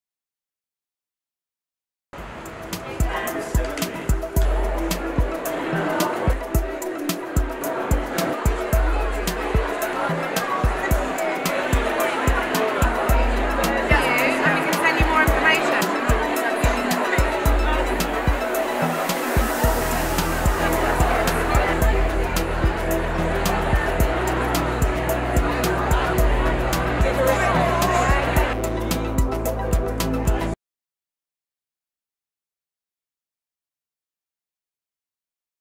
Miss Jones Showcase
Following up to the success of previous showcases, Miss Jones presents our latest showcase, hosted in the Ballroom at Curtain Hotel on the 26th of February. Giving you the opportunity to network with 25+ of the hottest new venues, restaurants and services and network other PAs over some drinks and nibbles and be in the chance of winning some great prizes!